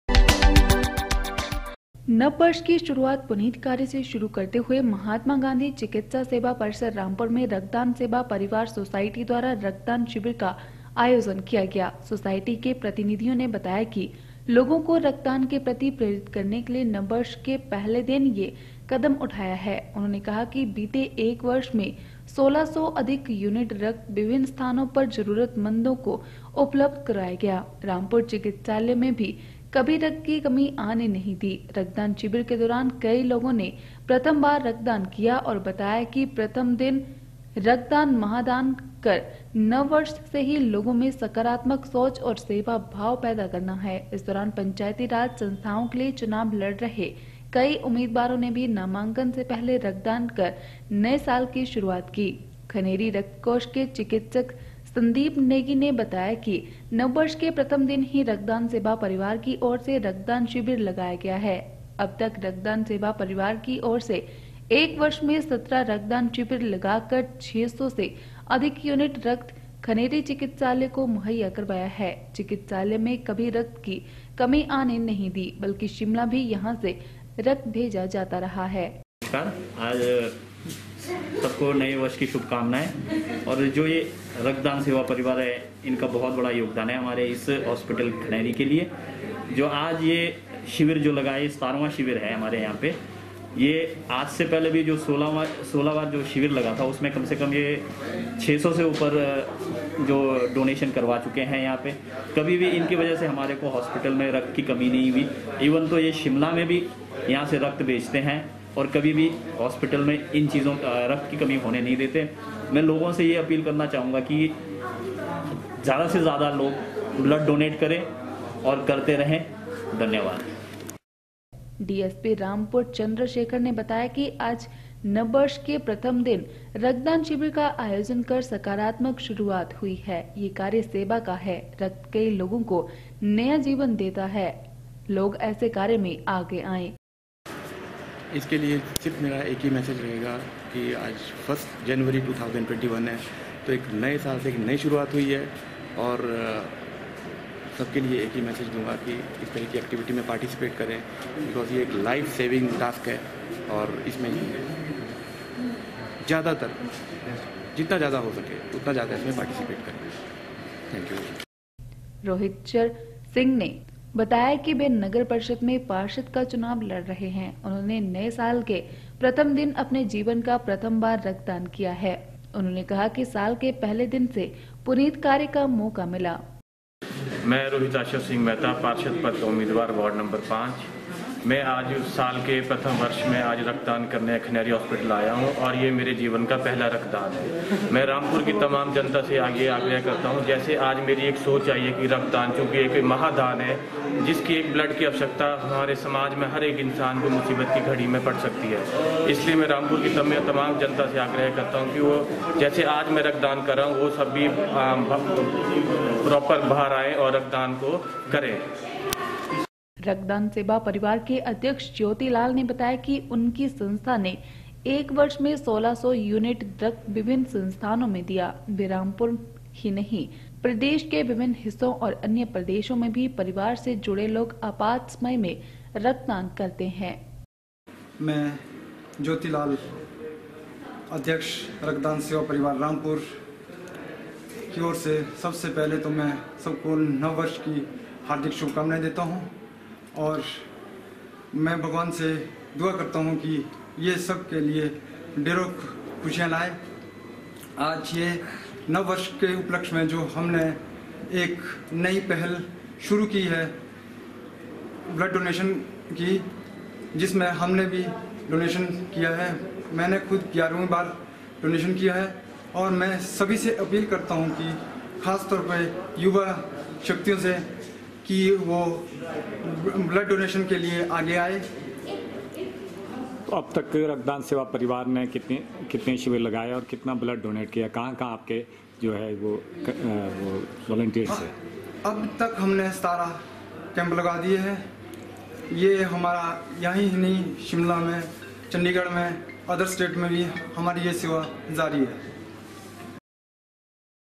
2 jan 2 रामपुर ने नय वर्ष की शुरुआत कीरक्तदान से
नव वर्ष की शुरुआत पुनीत कार्य से शुरू  करते हुए महात्मा गाँधी
चिकित्सा सेवा परिसर  रामपुर  में रक्तदान सेवा परिवार सोसाइटी द्वारा
रक्तदान शिविर का आयोजन किया । सोसाइटी  के प्रतिनिधियों ने  बताया की
लोगो  को रक्तदान के प्रति प्रेरित करने के लिए नव वर्ष के पहले दिन यह
कदम उठाया है।  उन्होंने कहा बीते एक वर्ष में सोलह सौ अधिक यूनिट रक्त
विभिन्न स्थानों पर जरुरतमंदो कोउपलब्ध कराया गया।